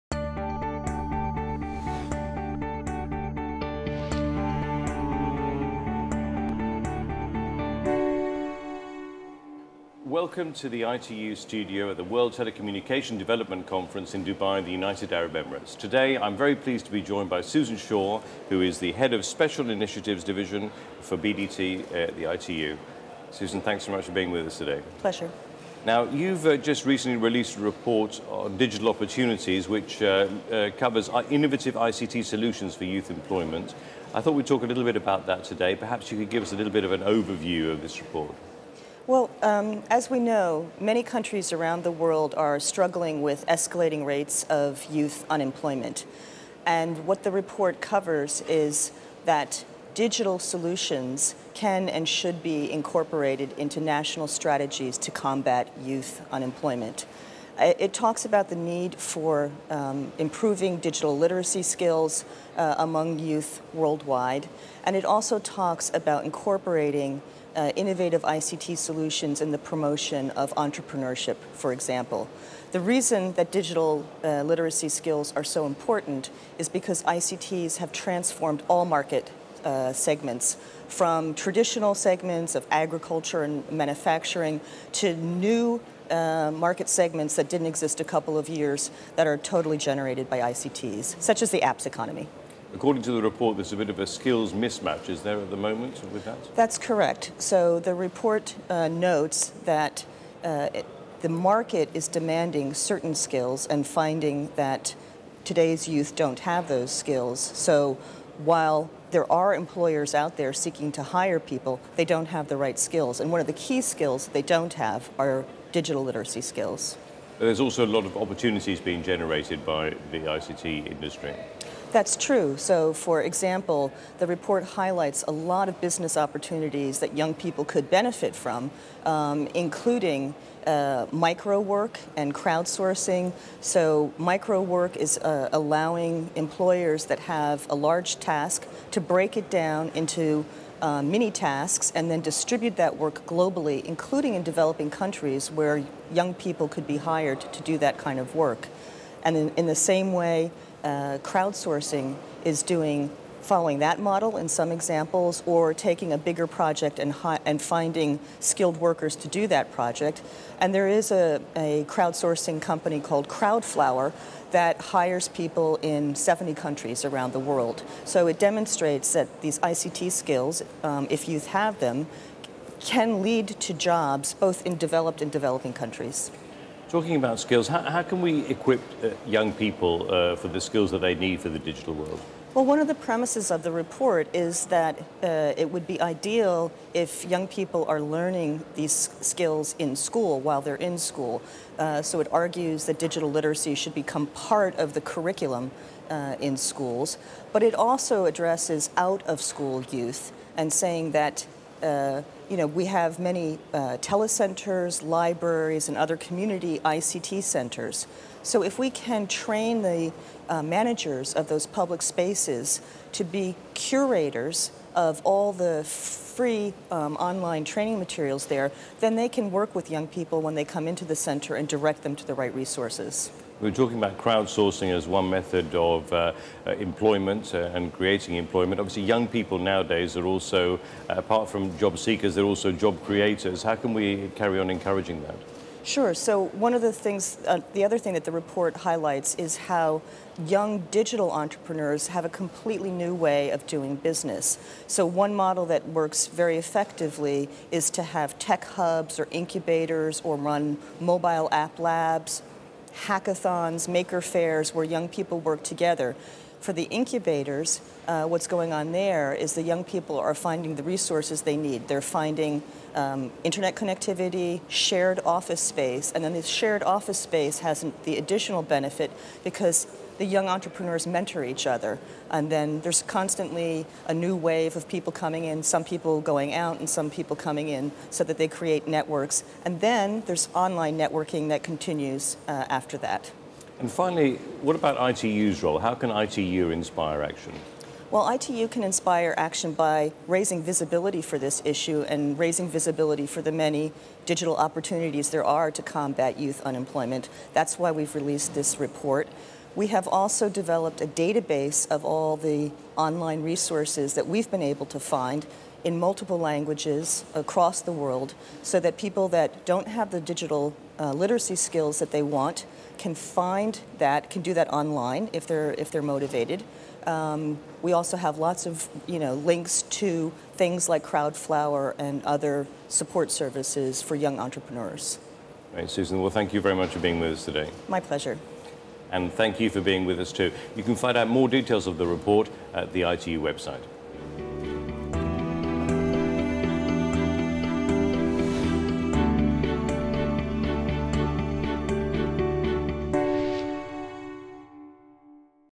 WTDC-14 INTERVIEWS: Susan Schorr, Head, Special Initiatives Div., BDT, ITU
Interview with Susan Schorr, Head of Special Initiatives Division, Telecommunication Development Bureau (BDT), ITU, at WTDC-14, 31 March 2014.